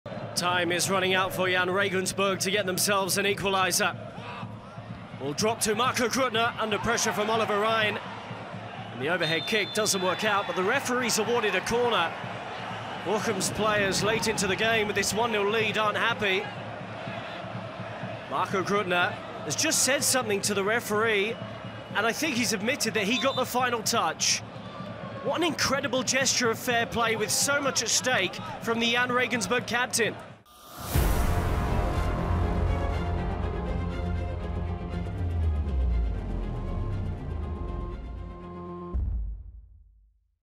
Fair Play Award - Despite Trailing in Injury Time
Marco Grüttner's grand gesture

It's the 90th minute of the game between Jahn Regensburg and VfL Bochum. The visitors are leading 1-0 so Regensburg throw everything they have forward. Grüttner tries an overhead kick inside the penalty area, but can only fire wide. A clear goal-kick. However, referee Dr. Martin Thomsen instead signalled for a corner. Regensburg captain Grüttner then stepped in to admit his touch, and the referee reversed his decision and awarded the goal-kick.